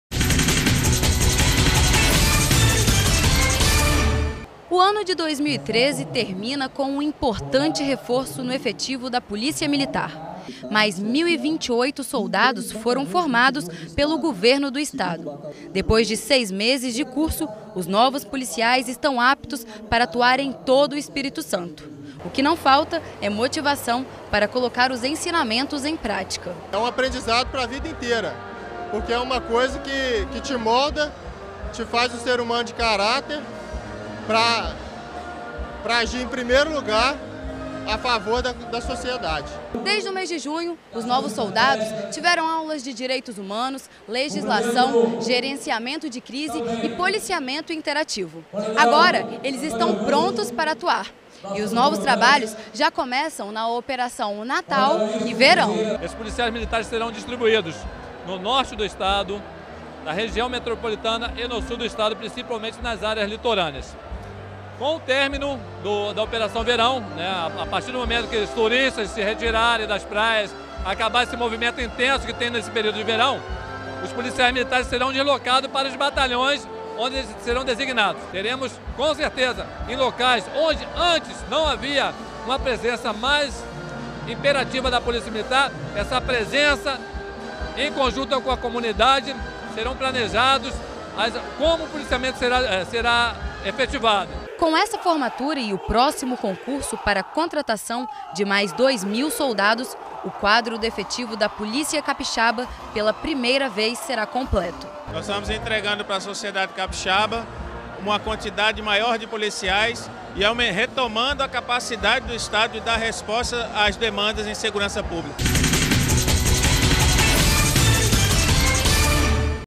Mais de mil soldados combatentes da PMES reforçarão a segurança no Estado
O salário para o cargo de soldado combatente é de R$ 2.631,97, incluindo gratificações. Durante o curso, os alunos soldados recebem remuneração de R$ 1.064,25.